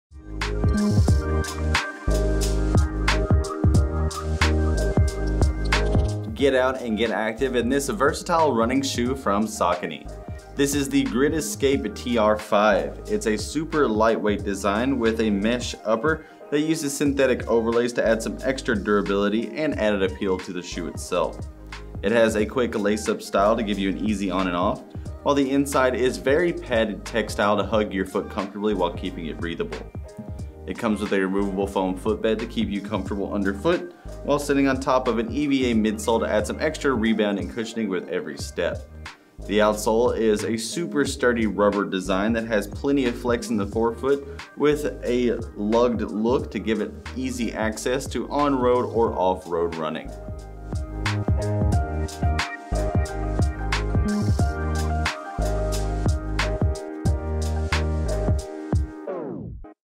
Saucony Grid Escape TR5 SKU: 9500107
Take on the roads in confidence with the Saucony&reg; Grid Escape TR5 running shoe.
Mesh fabric upper with synthetic overlays for structure and support.
Lace closure for a secure fit.
Cushioned insole and padded tongue and collar.
Shock absorbing midsole.
Rugged rubber outsole with aggressive tread for off-road traction.
Imported.